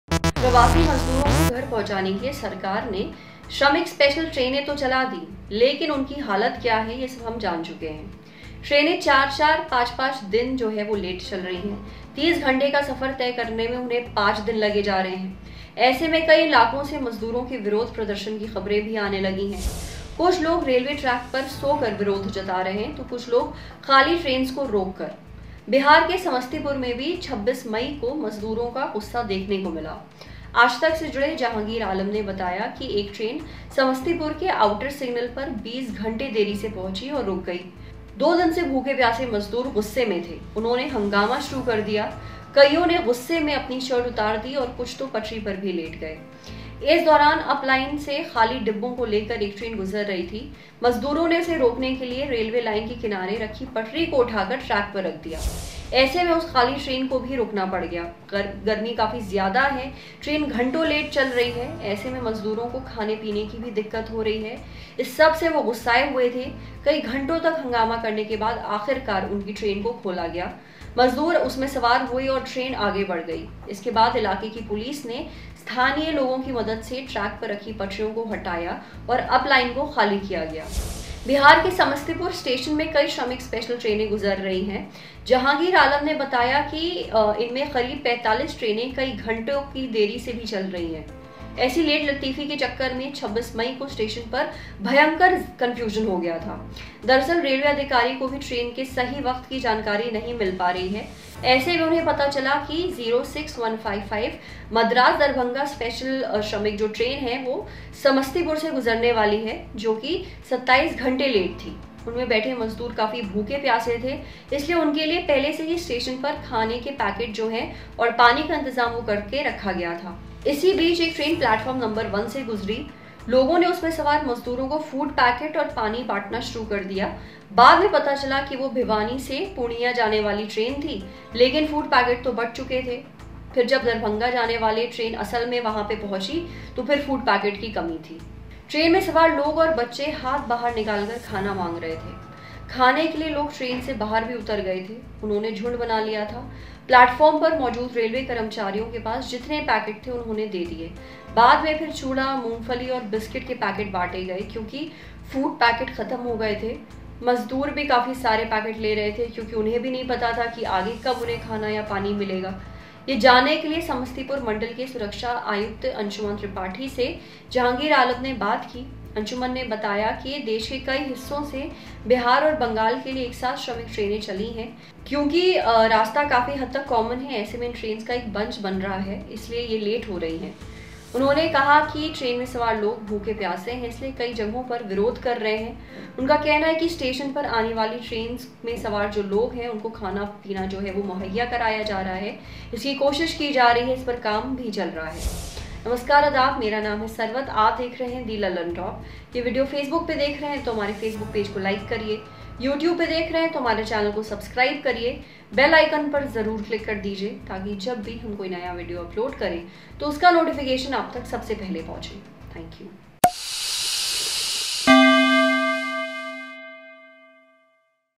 Lockdown 4.0 में Daily Wage Workers के लिए चल रही Shramik Trains का हाल बेहाल | Piyush Goyal
Daily wage workers travelling on Shramik Express passing through Samastipur train station staged protest. The train was running late by more than 20 hours and the workers were hungry. Watch the video for details.